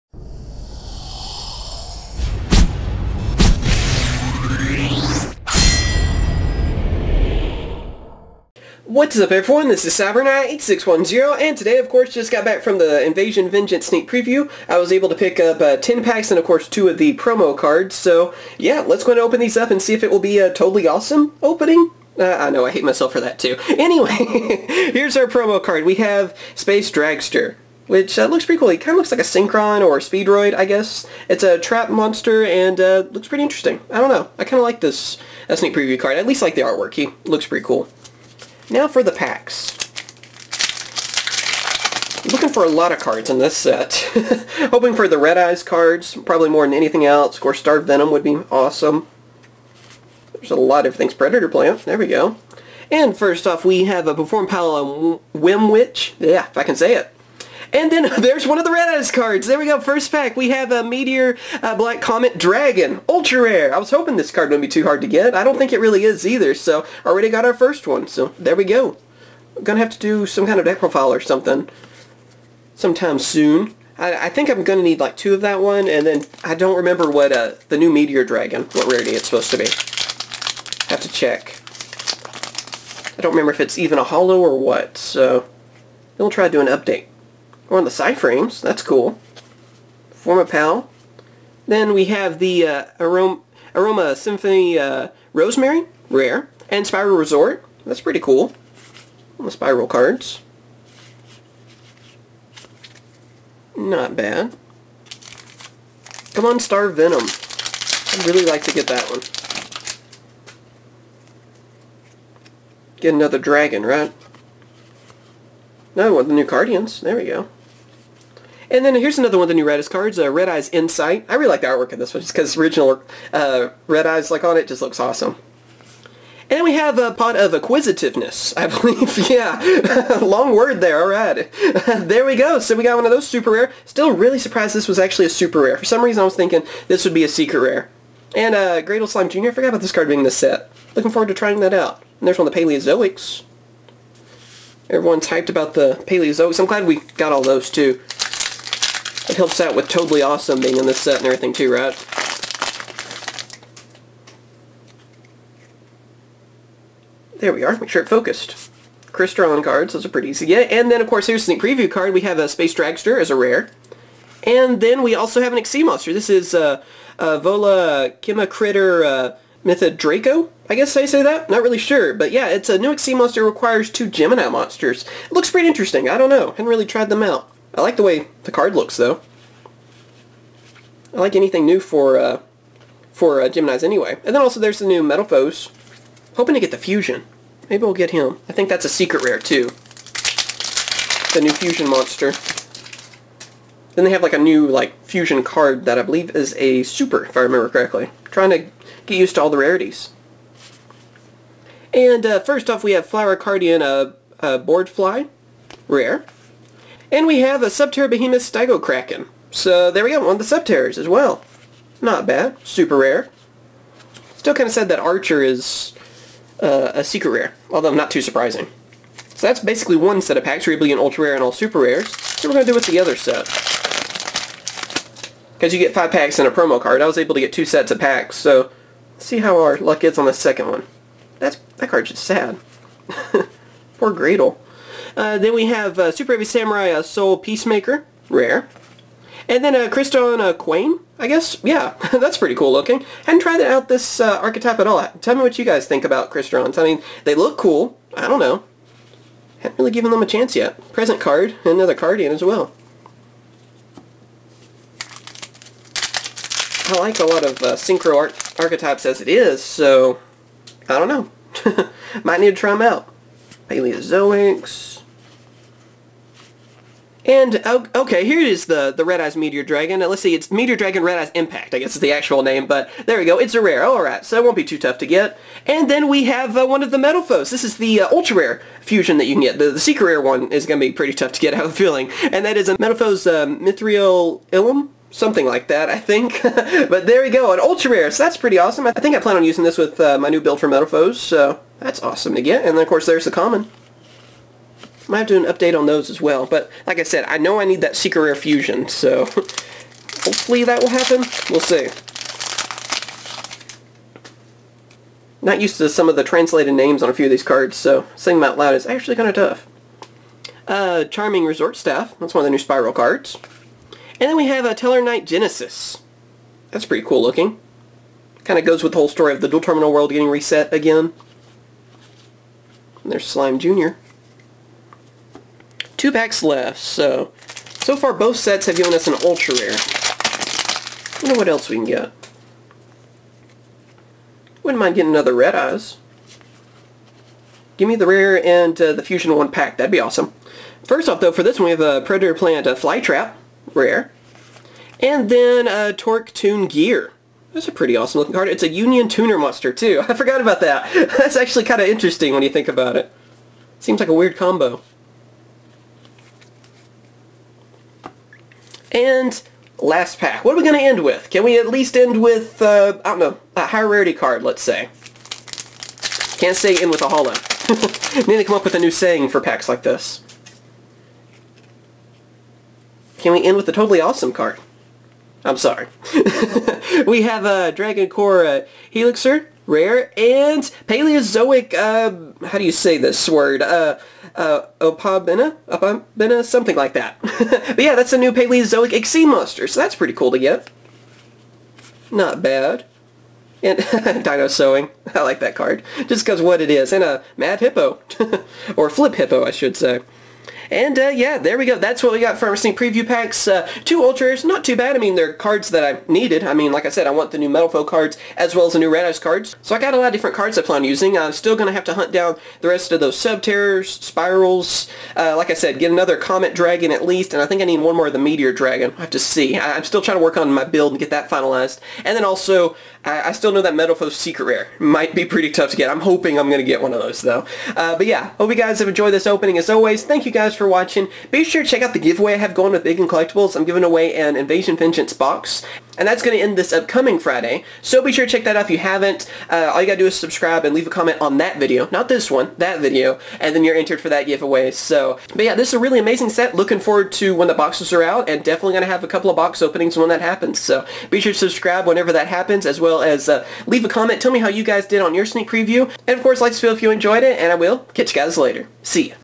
Yugioh Invasion: Vengeance Sneak Preview 10 Packs Opening
Here is my Yugioh Invasion: Vengeance Sneak Preview 10 Packs Opening. What other openings/ Yugioh videos do you guys want to see in the future? Hope you all enjoyed the video let's see if we can get 500 LIKES!